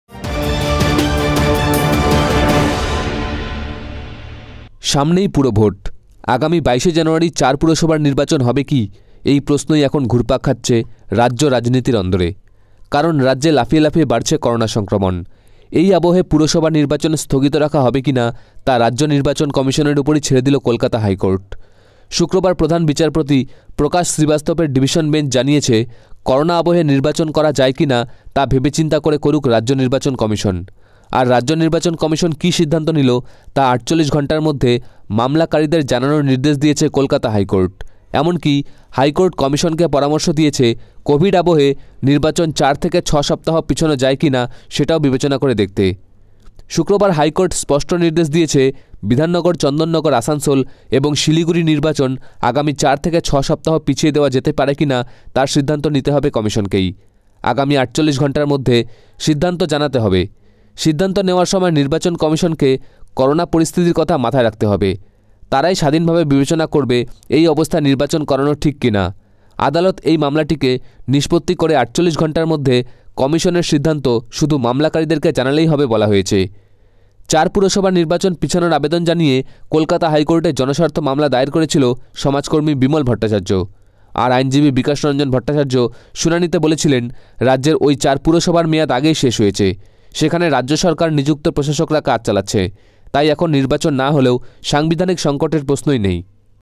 করোনা আবহে পুরভোট হবে কি? কমিশনকে ৪৮ ঘণ্টার মধ্যে জানানোর নির্দেশ কলকাতা হাইকোর্টের | bengalinews
করোনা আবহে পুরভোট হবে কি? কমিশনকে ৪৮ ঘণ্টার মধ্যে জানানোর নির্দেশ কলকাতা হাইকোর্টের | bengalinews | new2newz